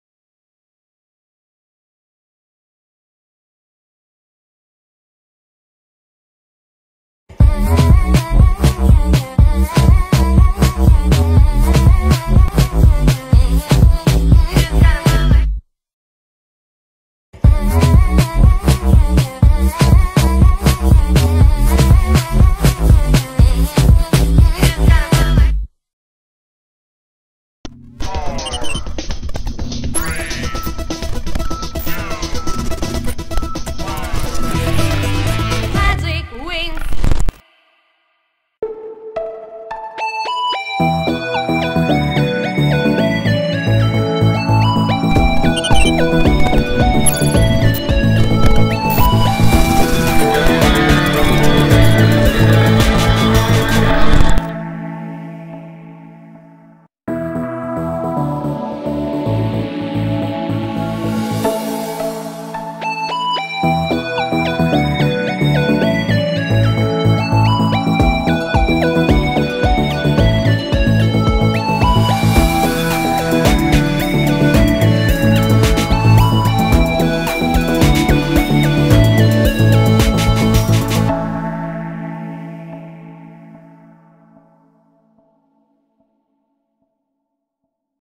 Channel Intros through the years - 9 Years of WCD Channel
On December 24th 2013 I uploaded my first video.
Nine years later, this little hobby of mine is still going strong.
Thank you all for the support through the years.
Happy Holidays and a Magical Year 2023.

-Sisco